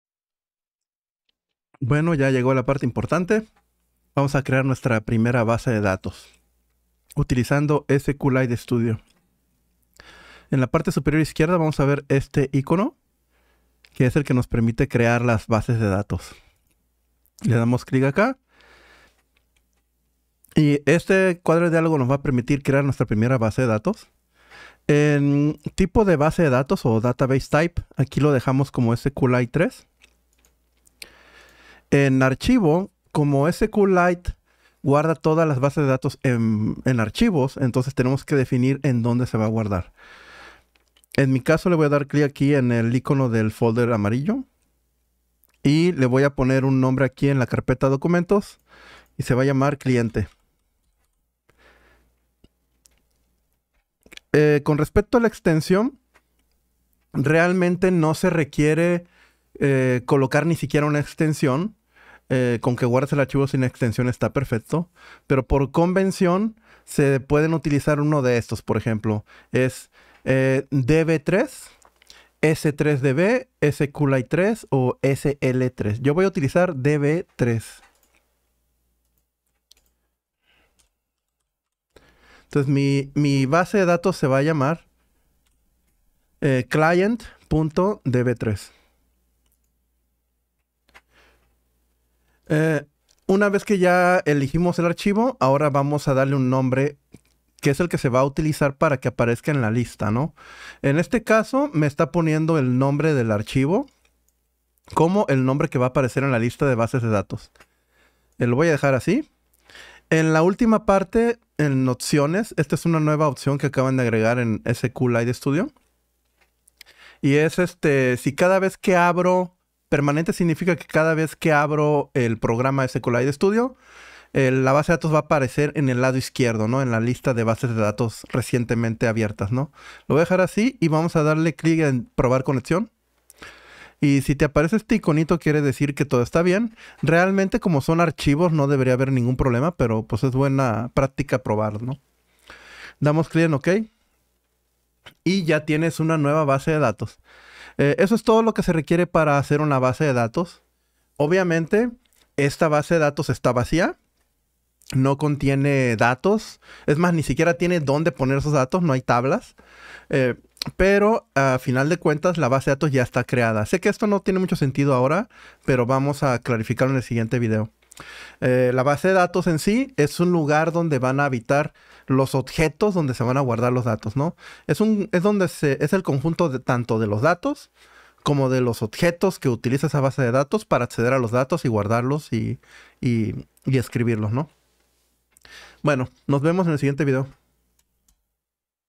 Crear una base de datos en SQLite studio
Las bases de datos en SQLite3 se guardan en archivos, la extension del mismo no importa, incluso puede no tener una, por convención usare nombre_base_datos.db3